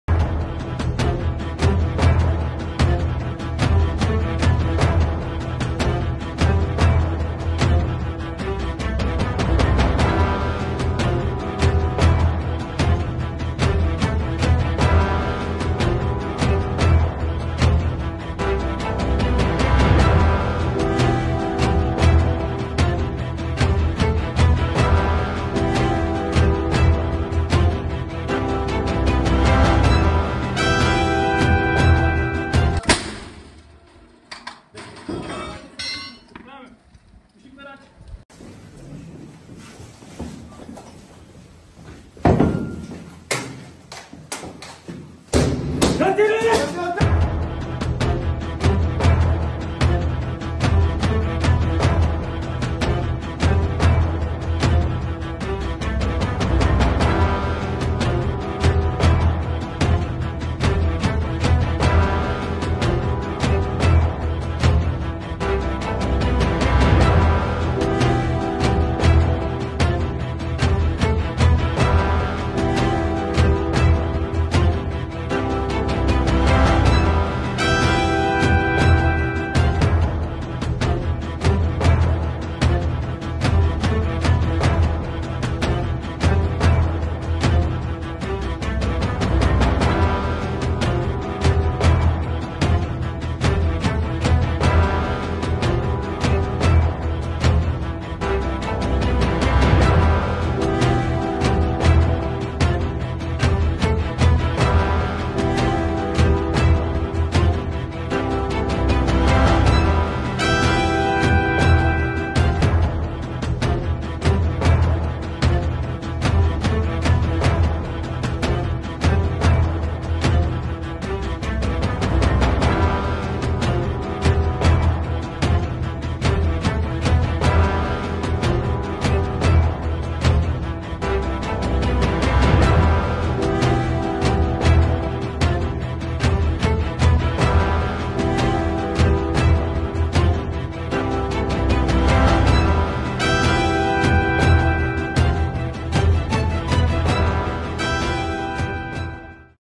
Malatya'da "NARKOGÜÇ-43" Operasyonu...
İçişleri Bakanı Ali Yerlikaya, "NARKOGÜÇ-43" Operasyonu'nda Malatya'nın da aralarında bulunduğu 45 ilde operasyon düzenledi.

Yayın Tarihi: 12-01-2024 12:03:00